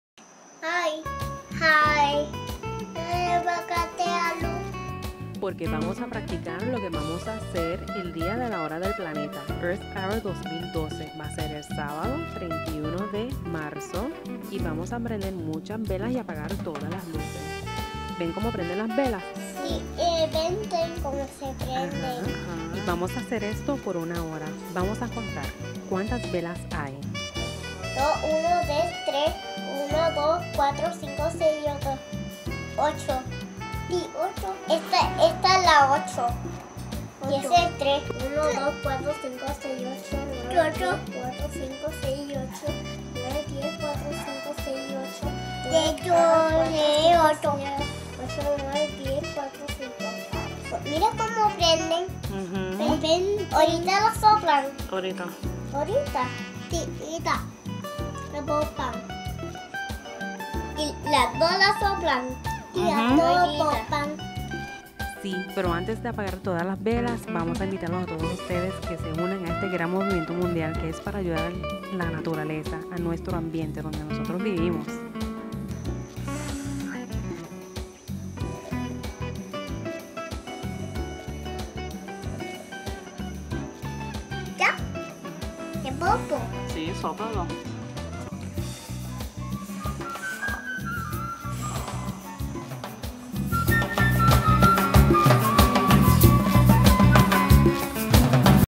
Niños Uniéndose a Earth Hour :::..
SUBSCRITE PARA NUEVOS VIDEOS ~ Earth Hour Puerto Rico

Earth Hour ~ La Hora del Planeta
Sábado, Marzo 31 de 2012
8:30pm - 9:30pm

Luces Apagadas por Una Hora, Movimiento Mundial para Ayudar el Medio Ambiente. :)

Lights Out for One Hour, Worldwide Movement to Help the Environment. :)